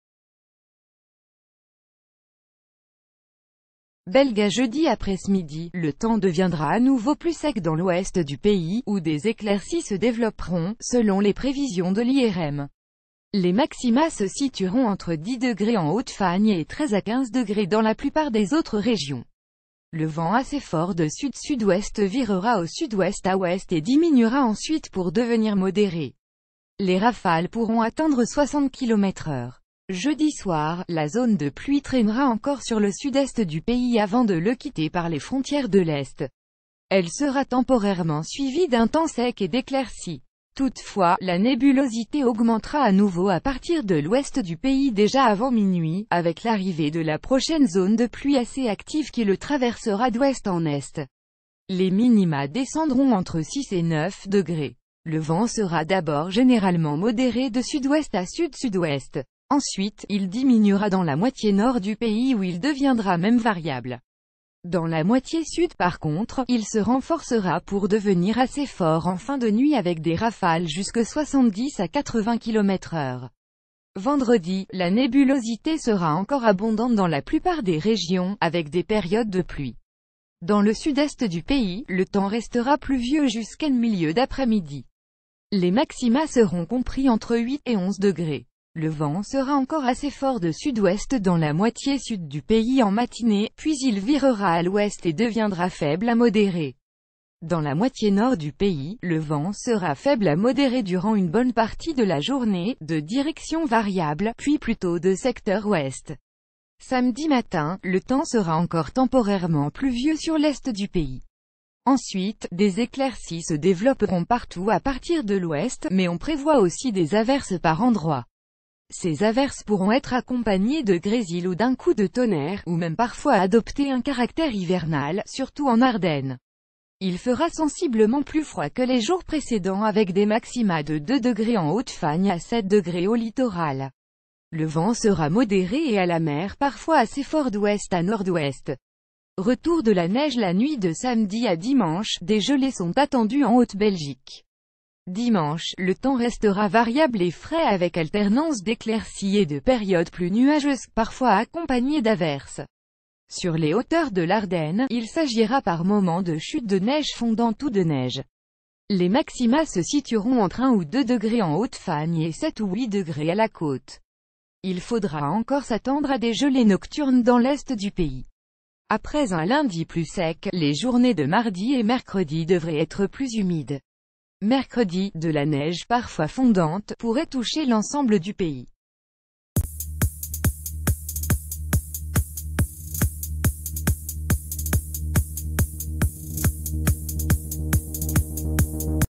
Dans son bulletin du jour, l'irm confirme l'arrivée de la neige sur tout le pays...
BelgaJeudi aprèsmidi, le temps deviendra à nouveau plus sec dans l’ouest du pays, où des éclaircies se développeront, selon les prévisions de l’IRM. Les maxima se situeront entre 10 degrés en Hautes F...